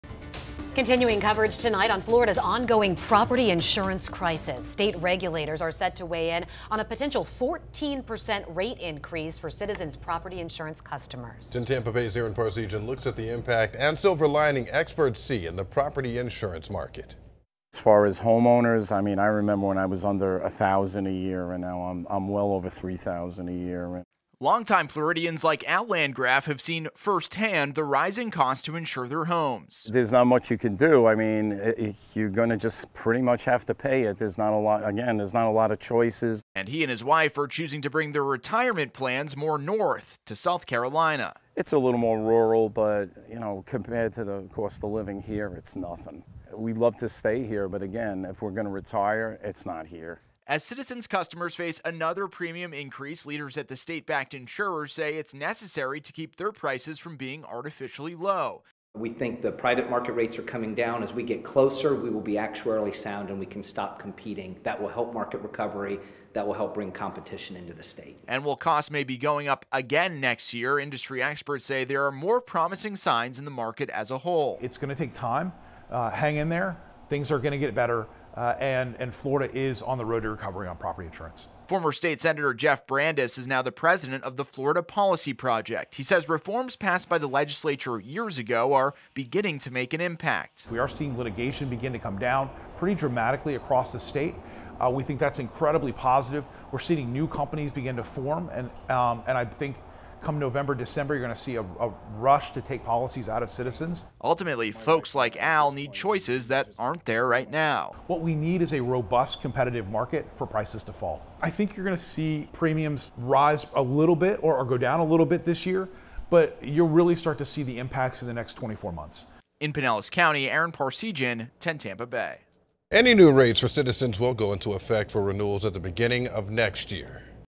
Florida homeowners face rising costs with Citizen Property Insurance
State regulators are set to weigh-in on a potential 14% rate increase for Citizens Property Insurance customers.